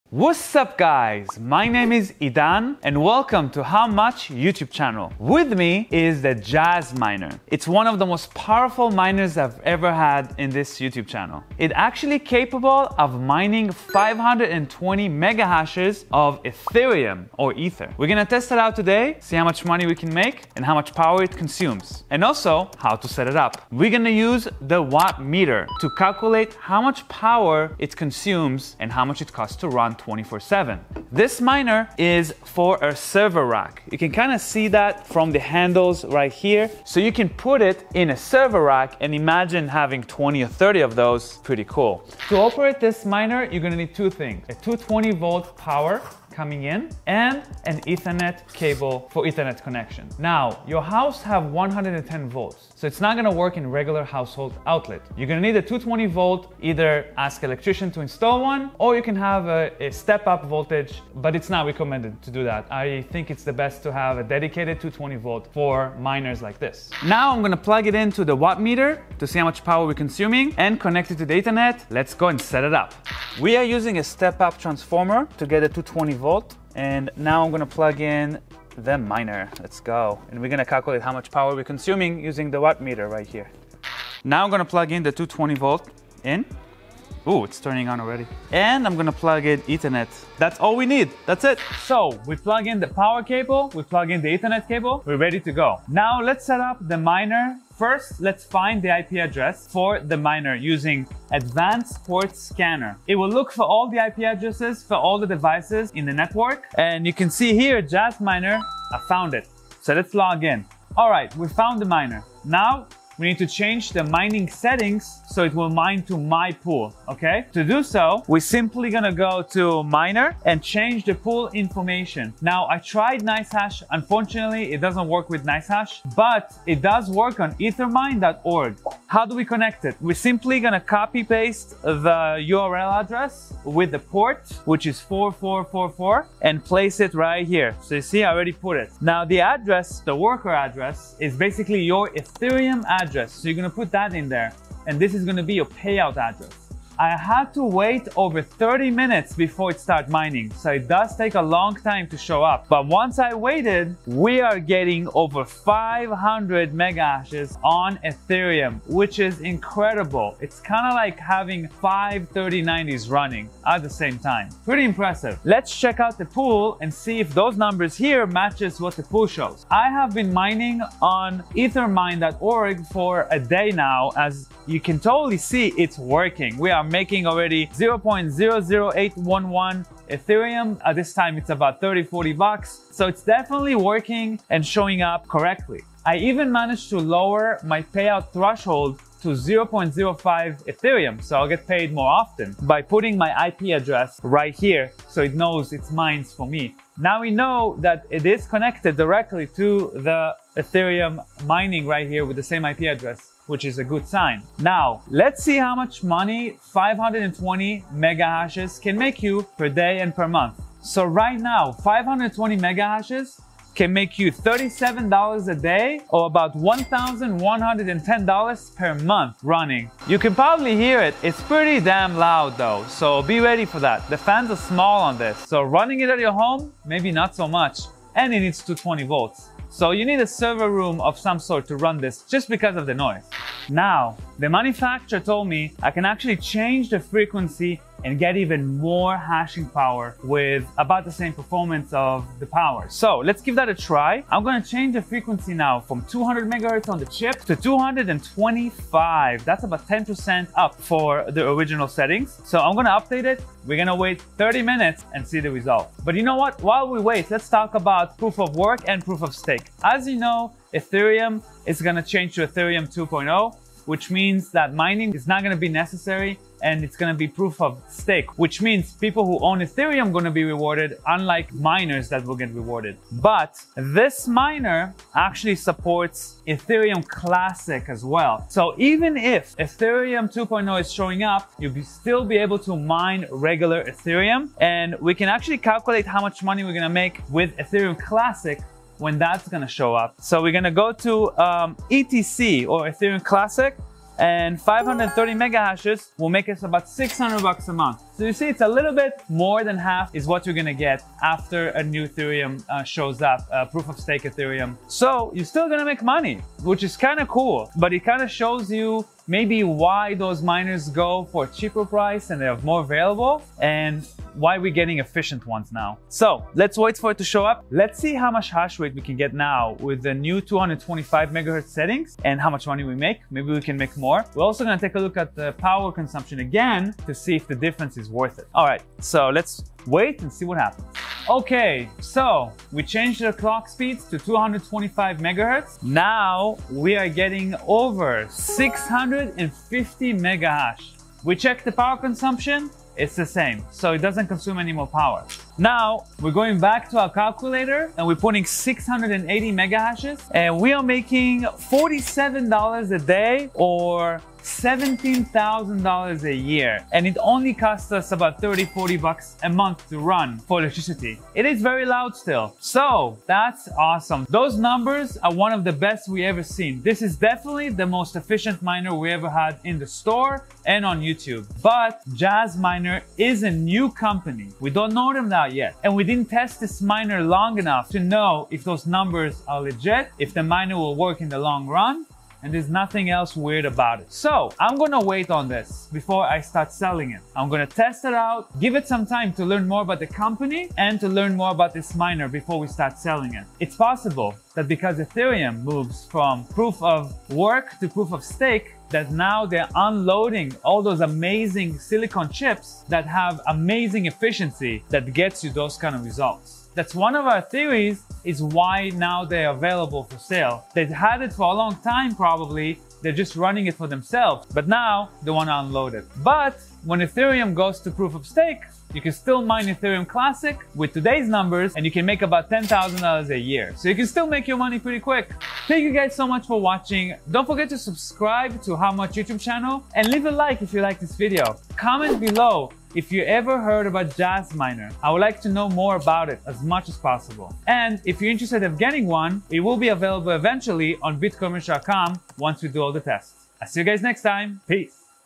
Ethereum Miner 600 MH/s at only 300W? Jasminer X4 Review
Buy your own Jasminer on my website (if it's in stock it will show up on the home page) -

In this video, I will review the Jasminer X4 and show you how much it makes per month and how much power it consumes.